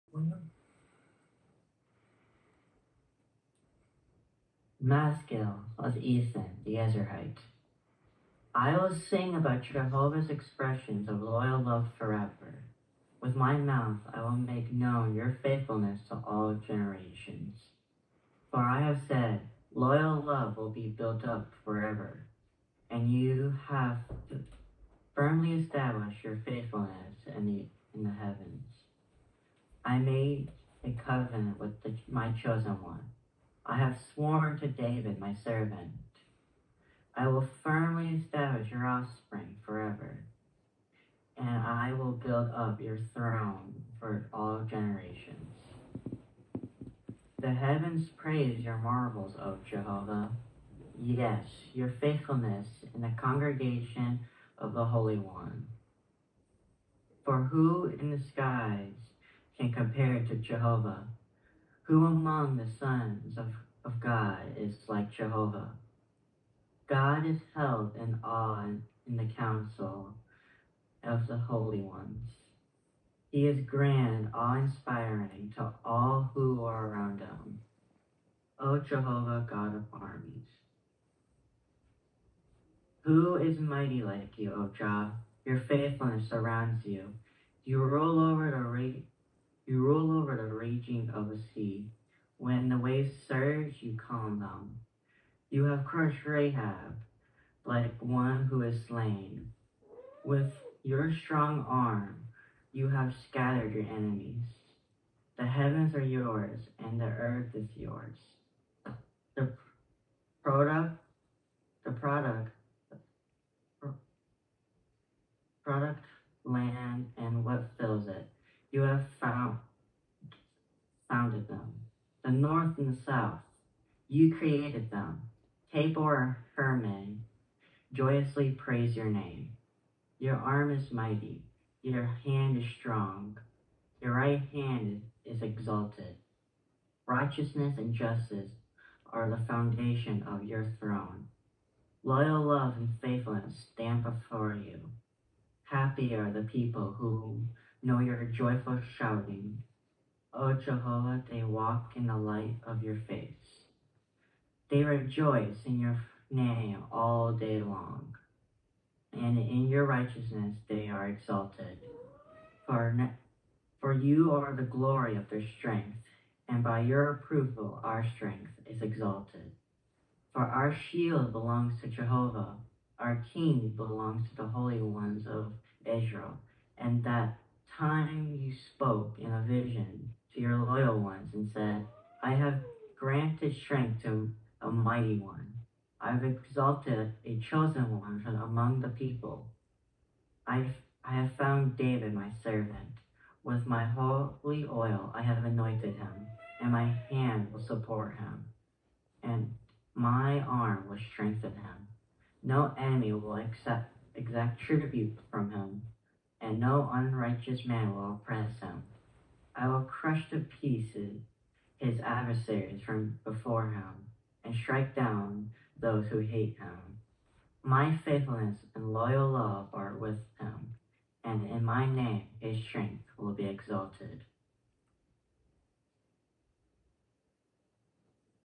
Micah reads Psalm 89:1-24
Tonight, we had the meeting over zoom due to the storm activity in the area.